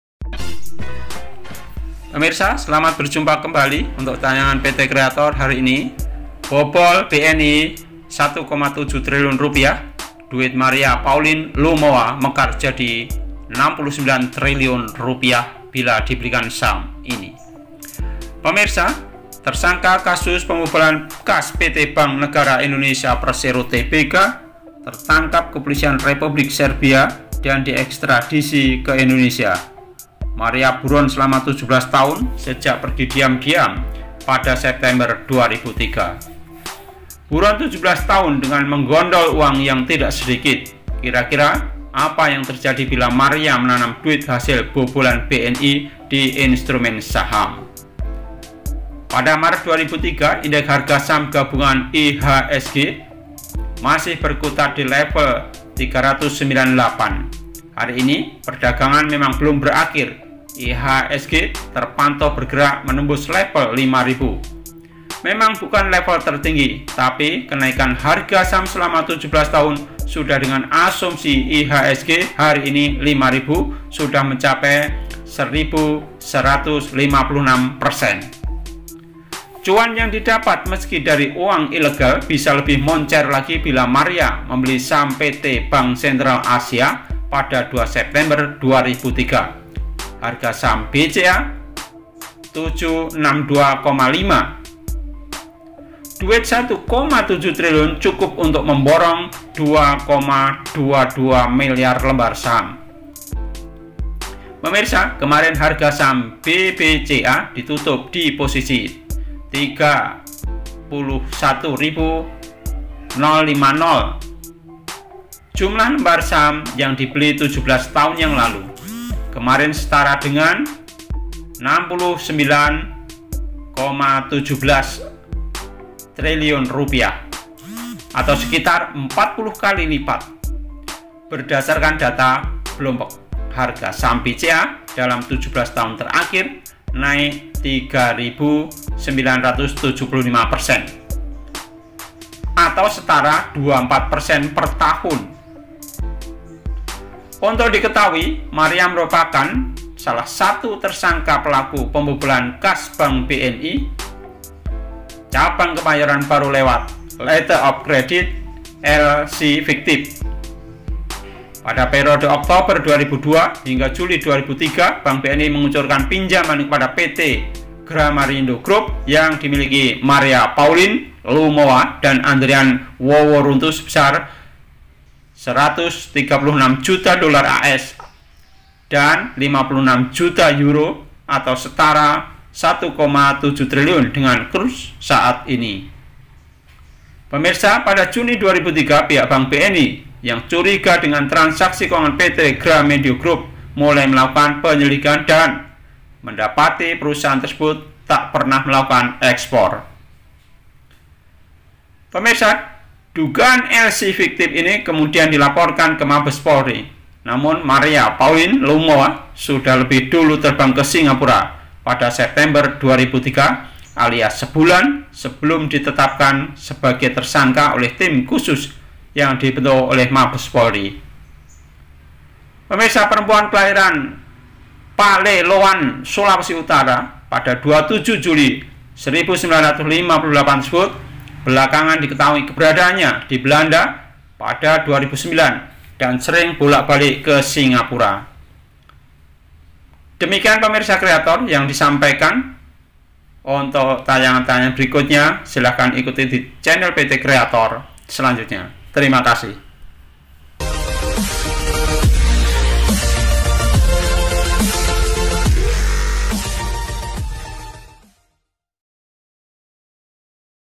Bobol BNI Duit Maria Lumowa Rp. 1,7 Triliun Bisa Jadi Rp.69 Triliun Selama 17 Tahun
Selama 17 tahun, Indeks Harga Saham Gabungan (IHSG) sudah mencetak kenaikan lebih dari 1.100 persen. Dana sebanyak Rp1,7 triliun bisa mekar menjadi lebih besar bila ditempatkan di instrumen saham.